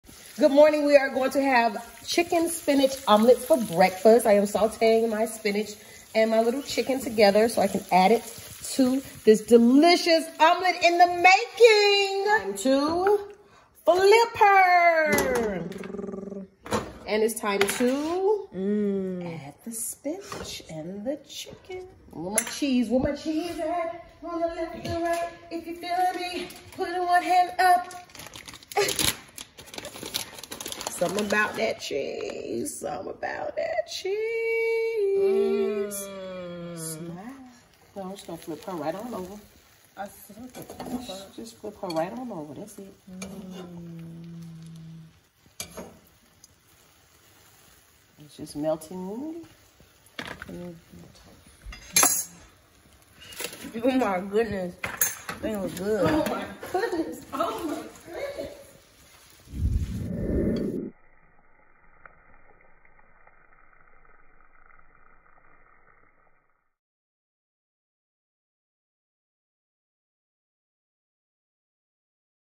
Xscape's Tamika Scott, Delicious Quick And Easy Cheesy Chicken & Spinach Omelette
~Cooking With Tamika Scott~
Follow Tamika Scott
Brolic Entertainment